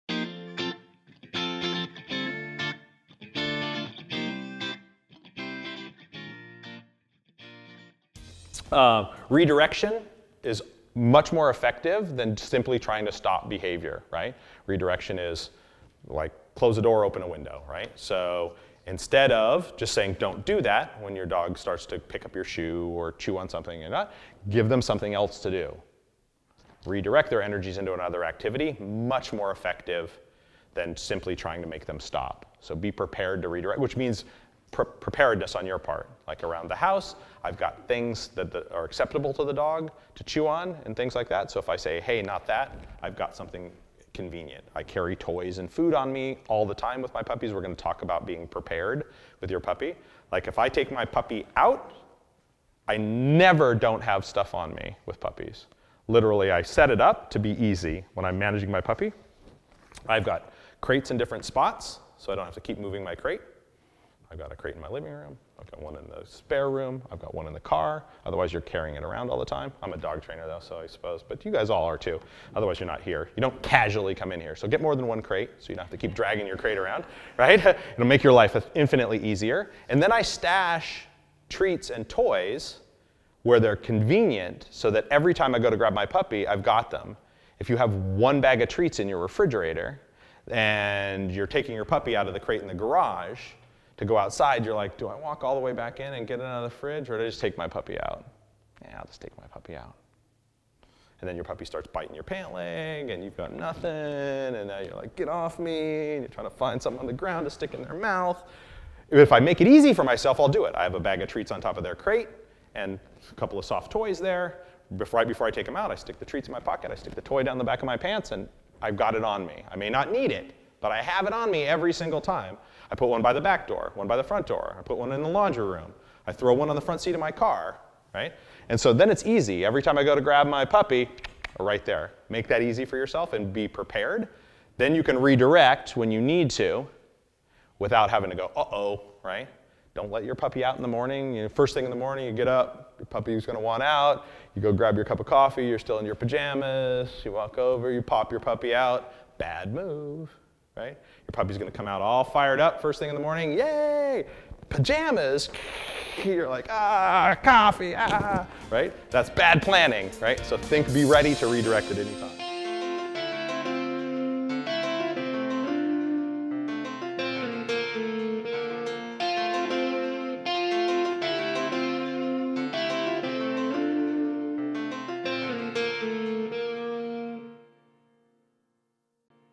Michael Ellis on Be Prepared To Redirect Your Puppy
This video was taken out of Leerburg’s Puppy Development Workshop online course with Michael Ellis.